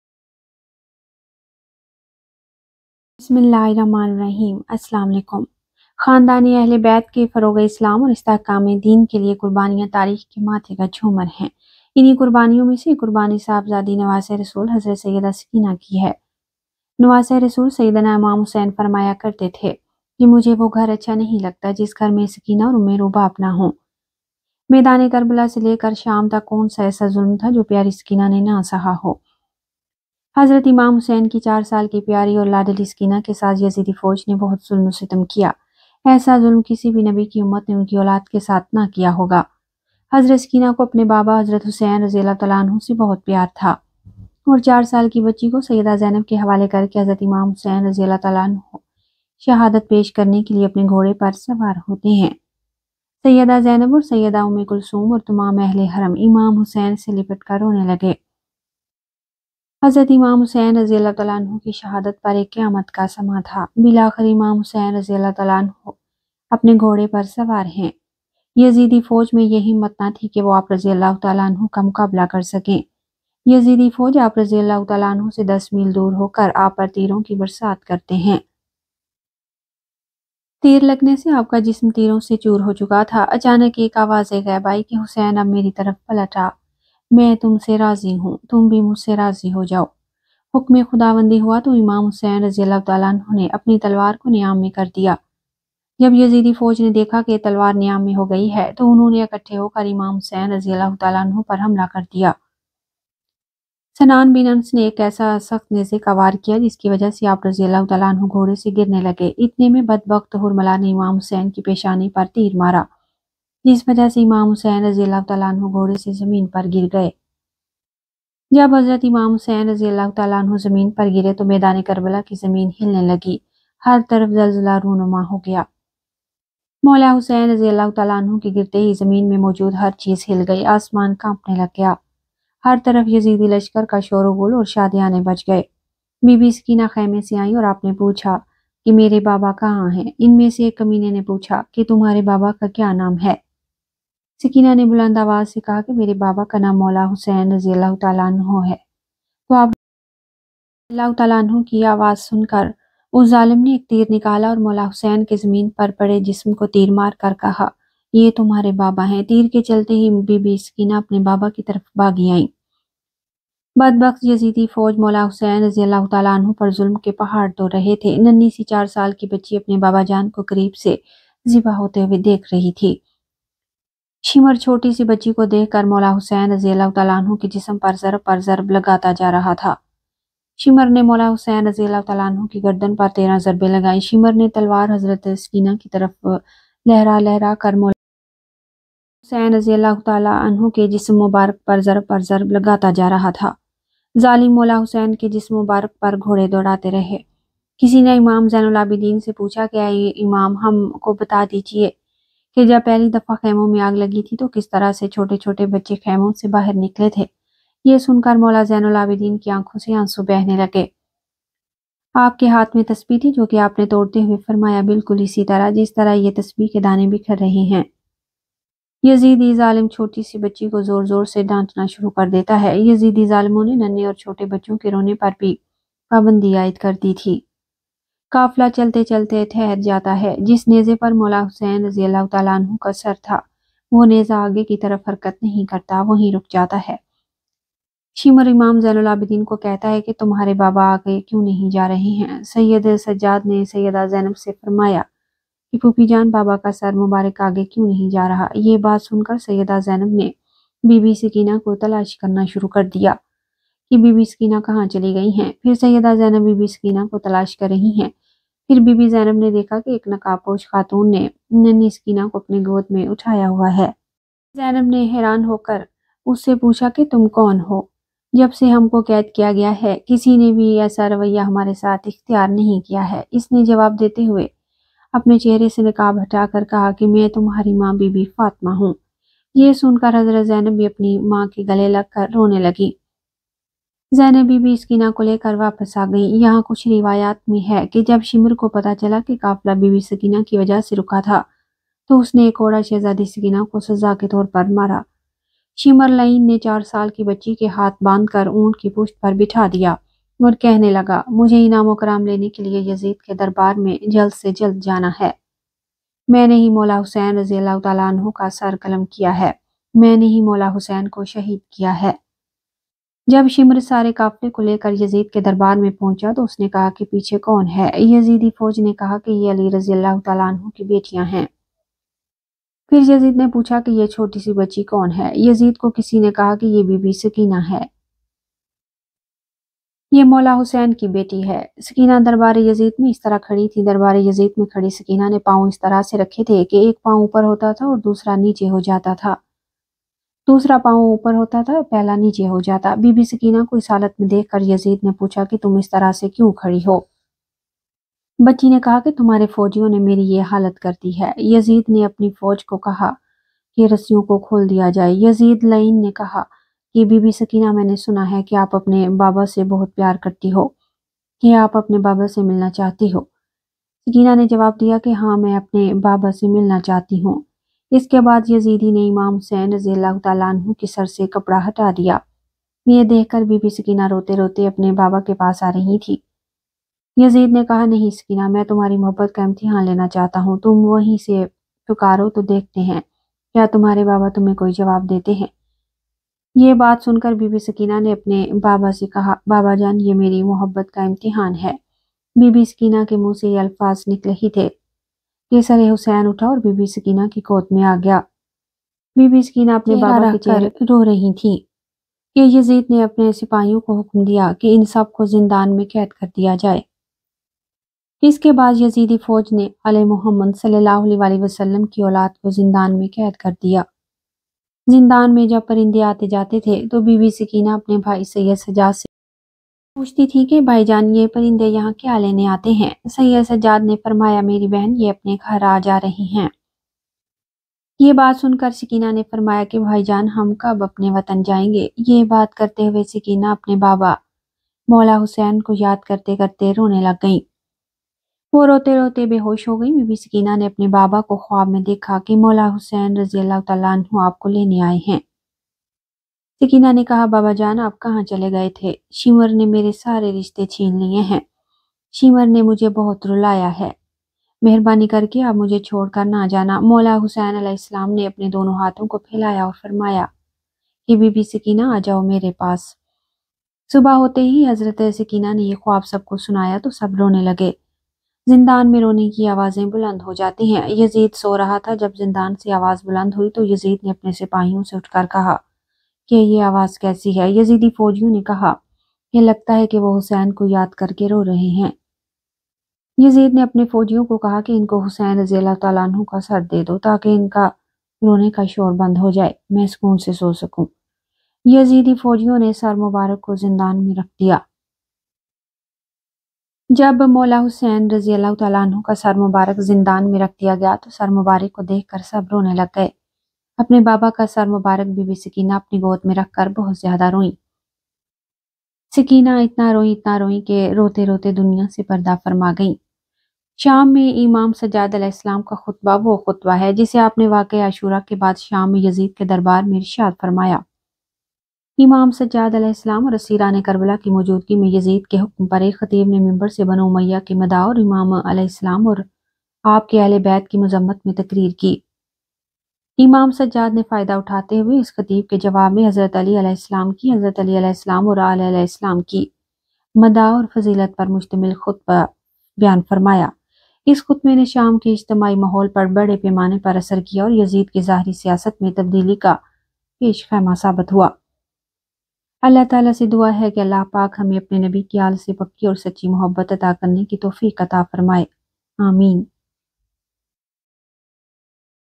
Hazrat Bibi Sakina Ki Shahadat Ka Waqia || Karbala Ka Waqia || 10 Muharram
Islamic stories in urdu hindi
Islamic stories in hindi
Urdu kahaniyan
Ikhlaqikahaniyan
Moralstories
Islamic

Voice: Noshii Gull
Label:Noshi_OfficialUK:
Editor:My'Self